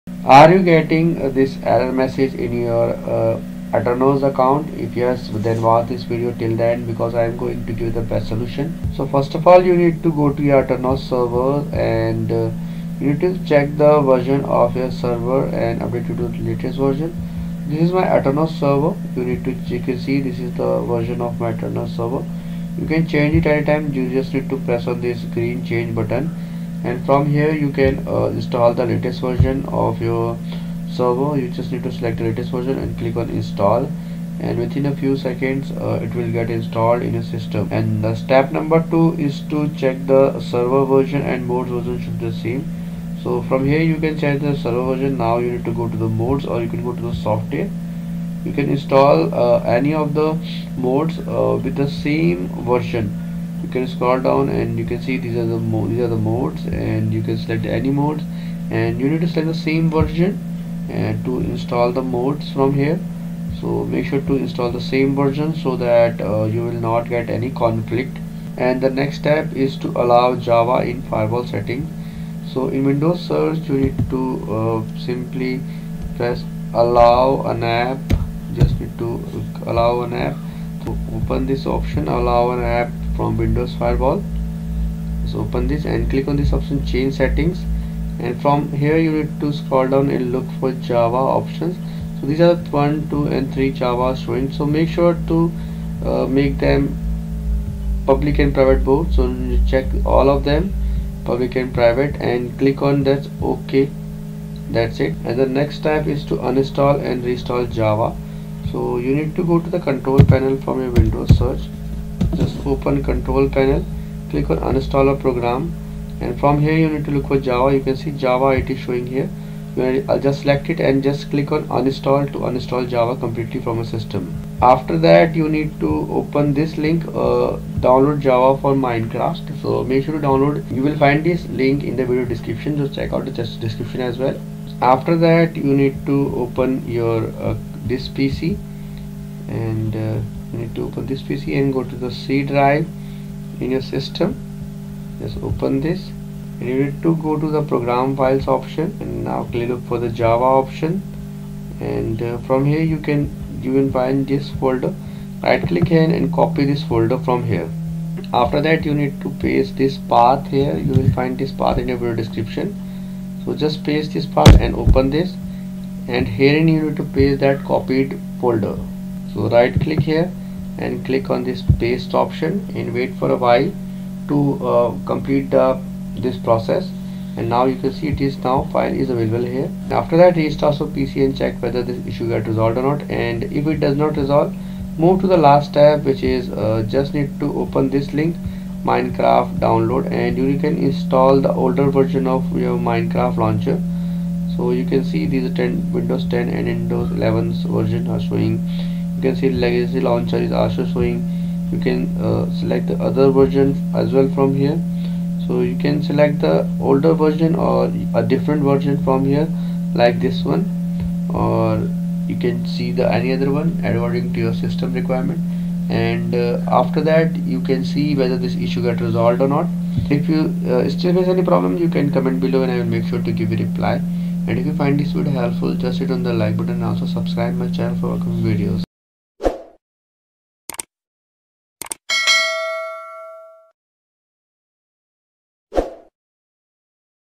[Error Fixed] Minecraft Login Finished Larger Than I expected, Found 1 Bytes Extra (Minecraft)
[Error Fixed] Minecraft Login Finished Larger Than I expected, Found 1 Bytes Extra (Minecraft, Aternos)

Are you getting the following error message in your Aternos server for Minecraft launcher:

Failed to connect to the server

Internal Exception: io.netty.handler.codec.DecoderException: java.io.IOException: Packet login/clientbound/minecraft:login_finished (ajz) was larger than I expected, found 1 bytes extra whilst reading packet clientbound/minecraft:login_finished

Here are some possible fixes which you can try out:

Step 1 – Update The Minecraft Server

Step 2 – Remember That The Version Both Your Minecraft Server and Installed Mods Should Be Same

Step 3 – Allow Java From The Firewall Settings

• In your system, find “Windows Defender Firewall”
• Choose “Allow an app or feature through Windows Defender Firewall”
• Select “Change Settings”
• Ensure that all the java fields should be check Private and Public
• Click on OK

Step 4 - Uninstall and Re-Install Java

• Go to Control Panel and then you need to uninstall Java
• Note: Check the system type in My Computer by right clicking over it before installing
• Install Java in your PC
• Now go to this path: This PC – C Drive – Program Files – Java – Copy folder name “jre1.8.0_51”
• Follow this path: C:\Users\Hp\AppData\Roaming\.tlauncher\starter\jre_default and paste the copied folder

Step 5 – Download and Install Older Version of Minecraft

Here is the link to download the older version of Minecraft launcher

Minecraft launcher error, aternos server error, java error in Minecraft launcher,